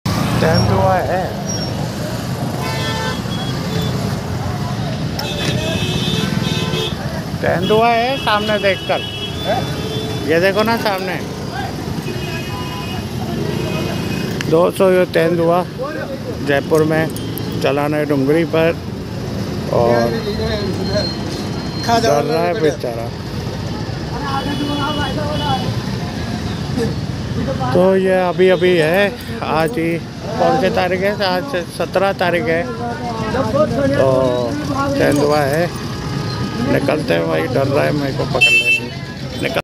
Jaipur City Jhalana Dungri Mein Dikha Tendua || Tenduaa dekhne Lagi Bheed
Products use

Maono au400 mic -

 Yuluz mic -

Motovlogging camera - GoPro8 black

Helmet SMK -

Mobile Narzo 20pro